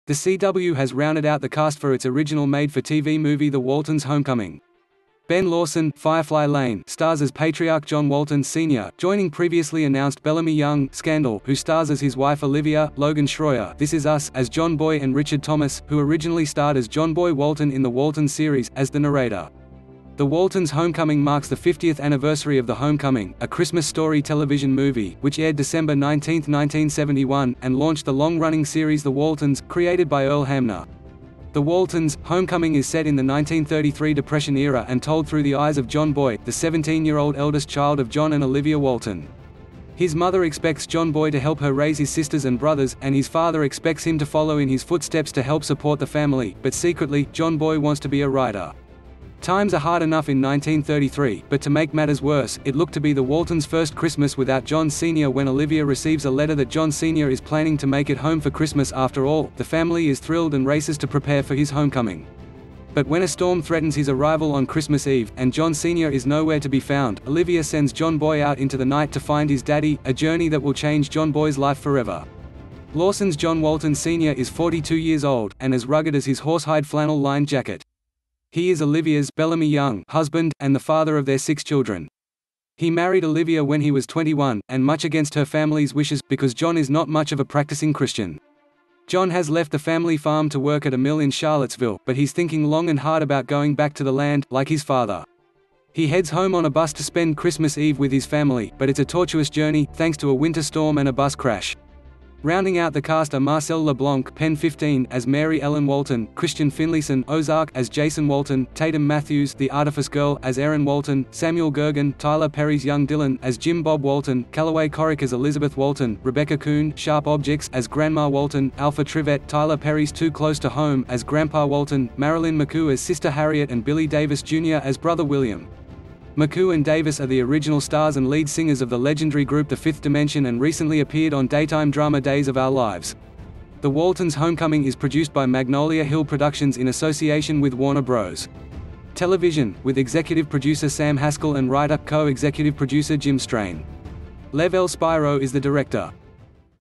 ‘The Waltons’ Homecoming’ Ben Lawson To Star As John Walton Sr In The CW
The CW has rounded out the cast for its original made-for-TV movie The Waltons’ Homecoming. Ben Lawson (Firefly Lane) stars as patriarch John Walton Sr., joining previously announced Bellamy Young (Scandal) who stars as his wife Olivia, Logan Shroyer (This is Us) as John Boy and Richard Thomas, who originally starred as John Boy Walton in The Waltons series, as “The Narrator. ”The Waltons’ Homecoming marks the 50th anniversary of The Homecoming: A Christmas Story television movie, which aired December 19, 1971, and launched the long-running series The Waltons, created by Earl Hamner. The Waltons: Homecoming is set in the 1933 Depression Era and told through the eyes of John Boy, the 17-year-old eldest child of John and Olivia Walton. His mother expects John Boy to help her raise his sisters and brothers, and his father expects him to follow in his footsteps to help support the family, but secretly, John Boy wants to be a writer. Times are hard enough in 1933, but to make matters worse, it looked to be the Waltons’ first Christmas without John Sr. When Olivia receives a letter that John Sr. is planning to make it home for Christmas after all, the family is thrilled and races to prepare for his homecoming. But when a storm threatens his arrival on Christmas Eve, and John Sr. is nowhere to be found, Olivia sends John Boy out into the night to find his daddy – a journey that will change John Boy’s life forever. Lawson’s John Walton Sr. is 42 years old, and as rugged as his horsehide flannel-lined jacket. He is Olivia’s (Bellamy Young) husband, and the father of their six children. He married Olivia when he was 21, and much against her family’s wishes, because John is not much of a practicing Christian. John has left the family farm to work at a mill in Charlottesville – but he’s thinking long and hard about going back to the land, like his father. He heads home on a bus to spend Christmas Eve with his family, but it’s a tortuous journey, thanks to a winter storm and a bus crash. Rounding out the cast are Marcelle Le Blanc (PEN 15) as Mary Ellen Walton; Christian Finlayson (Ozark) as Jason Walton; Tatum Matthews (The Artifice Girl) as Erin Walton; Samuel Goergen (Tyler Perry’s Young Dylan) as Jim Bob Walton; Callaway Corrick as Elizabeth Walton; Rebecca Koon (Sharp Objects) as Grandma Walton; Alpha Trivette (Tyler Perry’s Too Close To Home) as Grandpa Walton; Marilyn McCoo as Sister Harriet and Billy Davis Jr. as Brother William. McCoo and Davis are the original stars and lead singers of the legendary group The 5th Dimension and recently appeared on daytime drama Days of Our Lives. The Waltons’ Homecoming is produced by Magnolia Hill Productions in association with Warner Bros. Television, with executive producer Sam Haskell and writer/co-executive producer Jim Strain. Lev L. Spiro is the director.